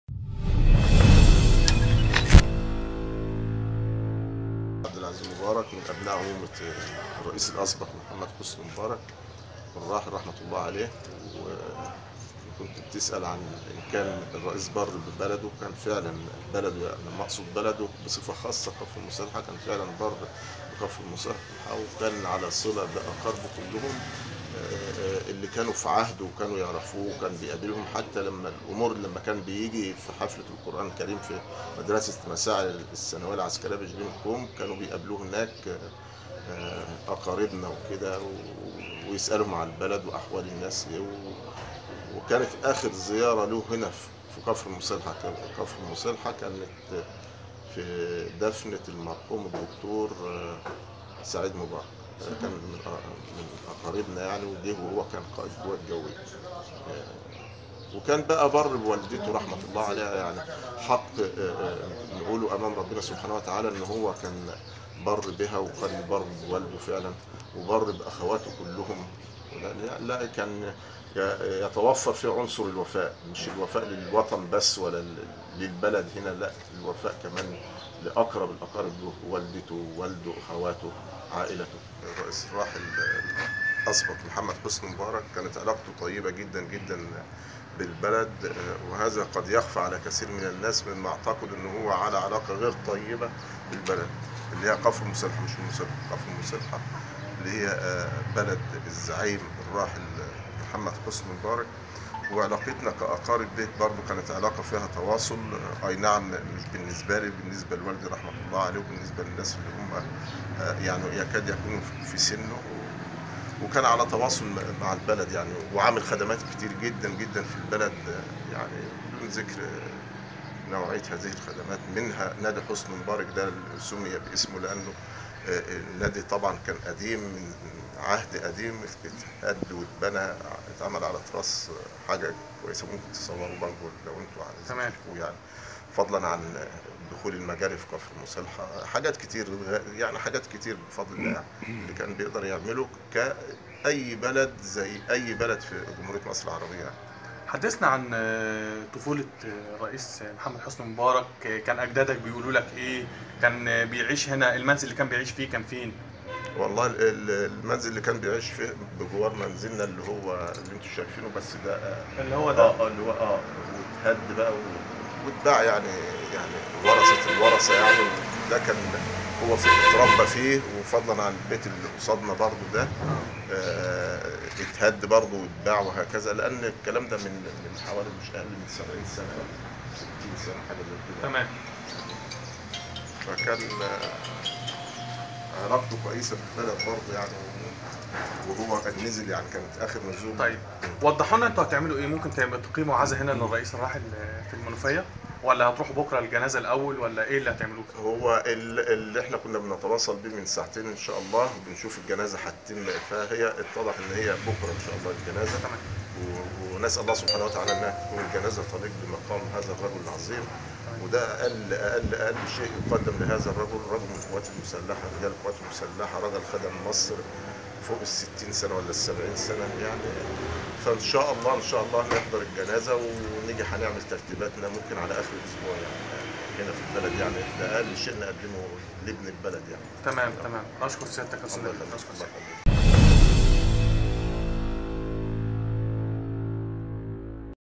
ابن عم الرئيس الأسبق "مبارك": كان حريصا على صلة الرحم
قال نبيل عبد العزيز مبارك، نجل عم الرئيس الأسبق محمد حسنى مبارك رئيس جمهورية مصر العربية، إنه تلقى خبر وفاته من التليفزيون المصرى بعد معاناته لسنوات من المرض